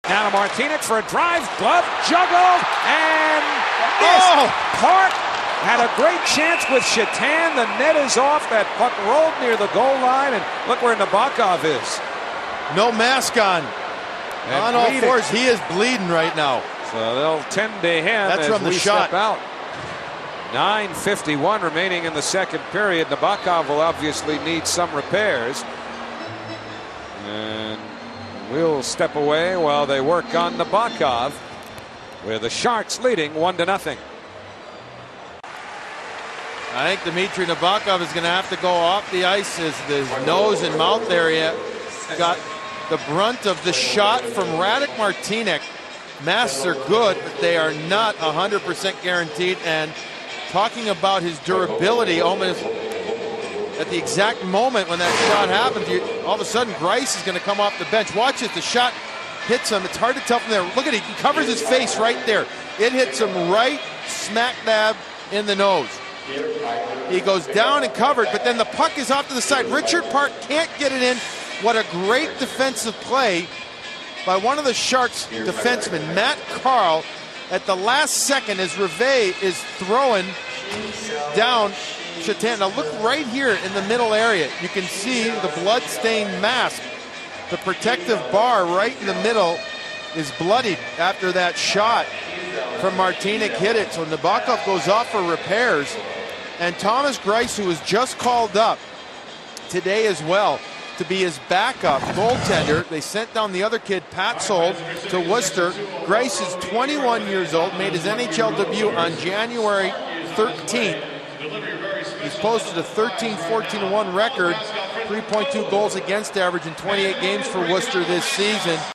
Puck Breaks Nabokov's Mask
Feb 18 2008 vs Islanders.

Couldn't get this video widescreen without destroying the quality.